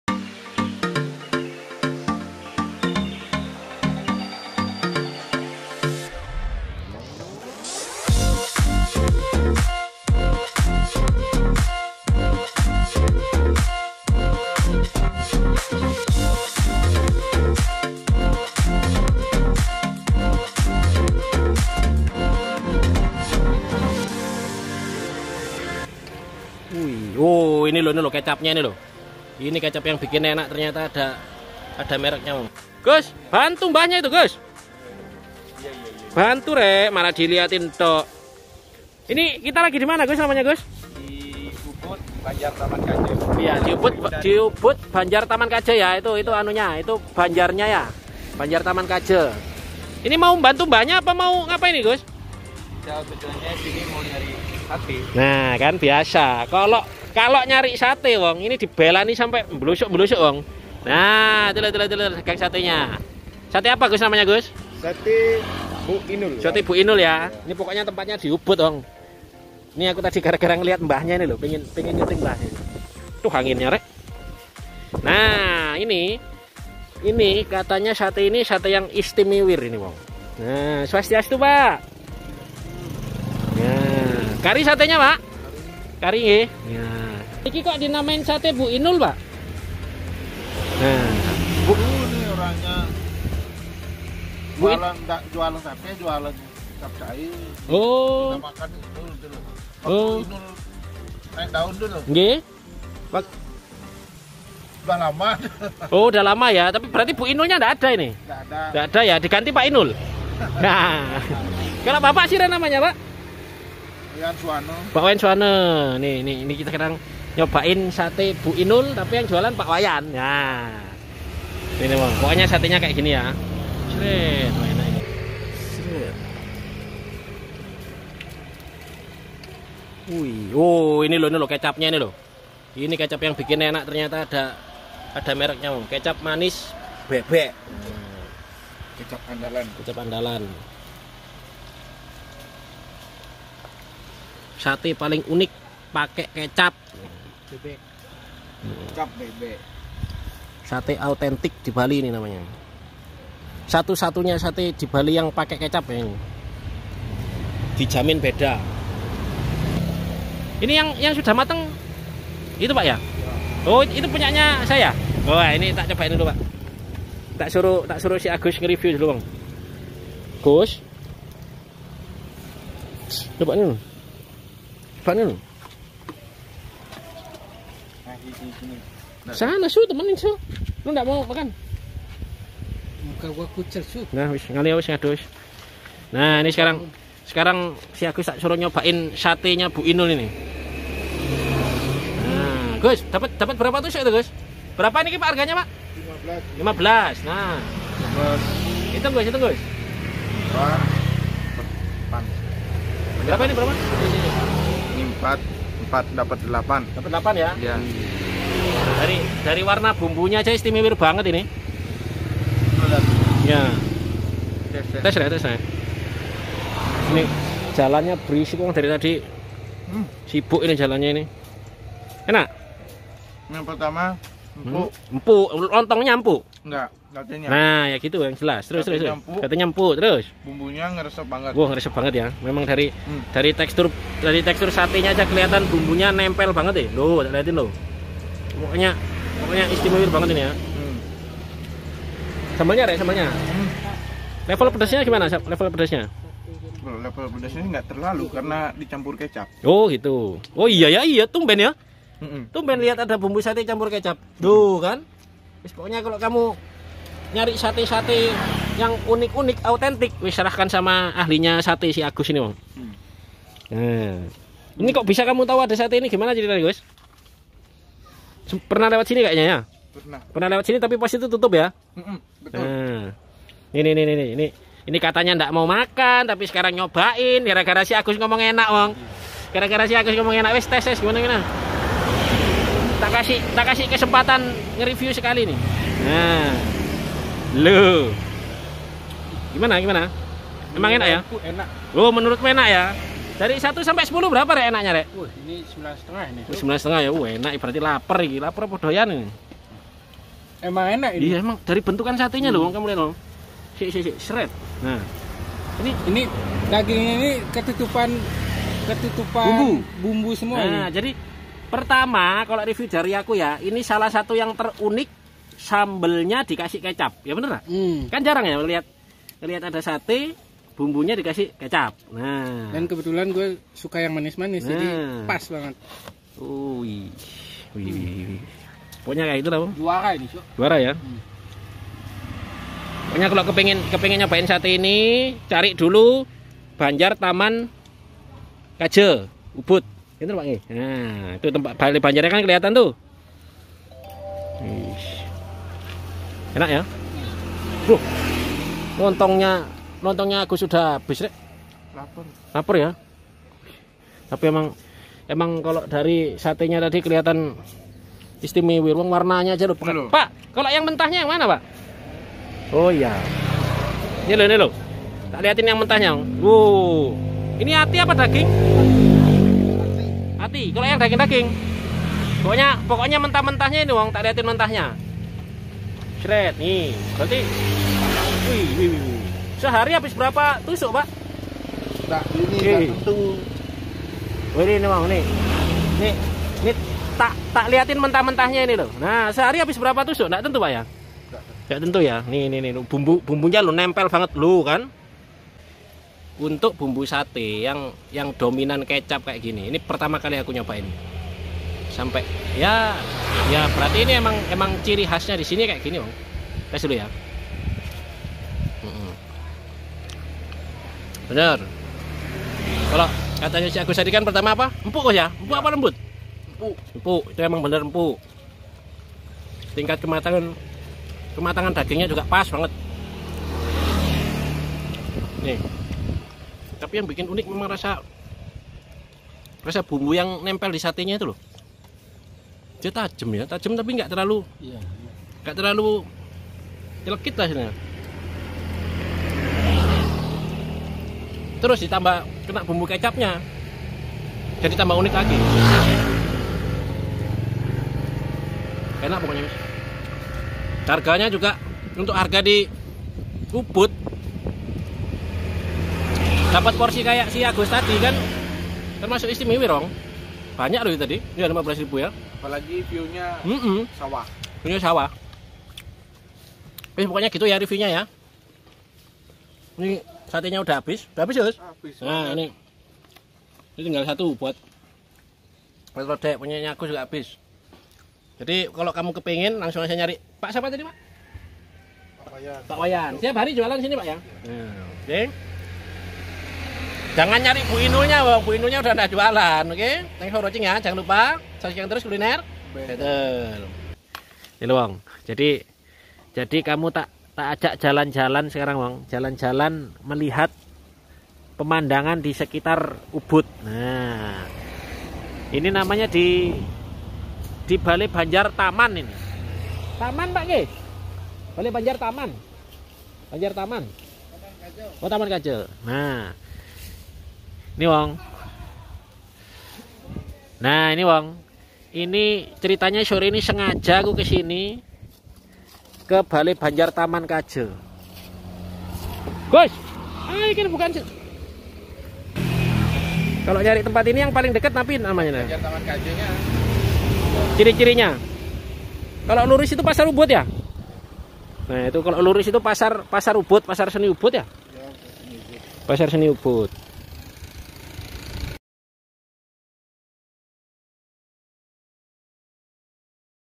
SATE PALING UNIK DI BALI || HANYA ADA DI SINI
SATE BU INUL
Lokasi : JL Sri Wedari UBUD depan banjar TAMAN KAJA
Buka jam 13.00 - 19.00 WITA
menu :
Sate Tipat
Harga Rp 15.000